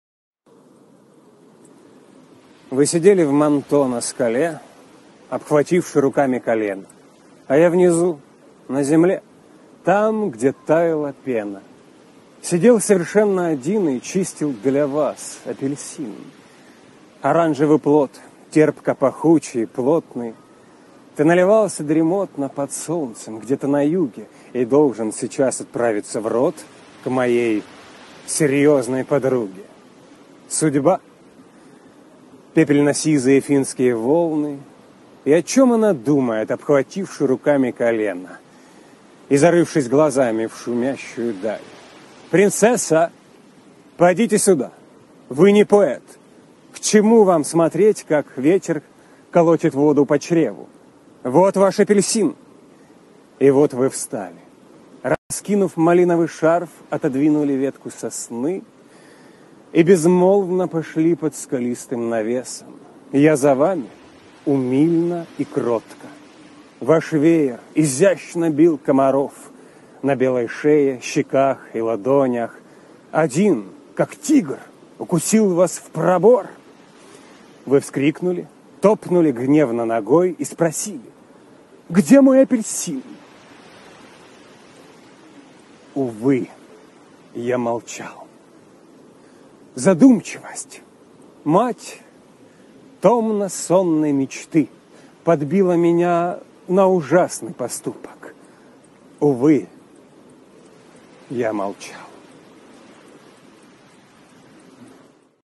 Апельсин I Автор стихотворения Саша Черный
Вы сидели в манто на скале,
Обхвативши руками колена.
А я — на земле,
Там, где таяла пена, —
Сидел совершенно один
И чистил для вас апельсин.
Оранжевый плод!
Терпко-пахучий и плотный...
Ты наливался дремотно
Под солнцем где-то на юге,
И должен сейчас отправиться в рот
К моей серьёзной подруге.
Судьба!
Пепельно-сизые финские волны!
О чём она думает,
Обхвативши руками колена
И зарывшись глазами в шумящую даль?
Принцесса! Подите сюда,
Вы не поэт, к чему вам смотреть,
Как ветер колотит воду по чреву?
Вот ваш апельсин!
И вот вы встали.
Раскинув малиновый шарф,
Отодвинули ветку сосны
И безмолвно пошли под скалистым навесом.
Я за вами — умильно и кротко.
Ваш веер изящно бил комаров —
На белой шее, щеках и ладонях.
Один, как тигр, укусил вас в пробор,
Вы вскрикнули, топнули гневно ногой
И спросили: «Где мой апельсин?»
Увы, я молчал.
Задумчивость, мать томно-сонной мечты,
Подбила меня на ужасный поступок...
Увы, я молчал!

@Саша Черный

1. Карта Сбербанка - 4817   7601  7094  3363
2. Яндекс Деньги -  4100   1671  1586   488
3. Qiwi - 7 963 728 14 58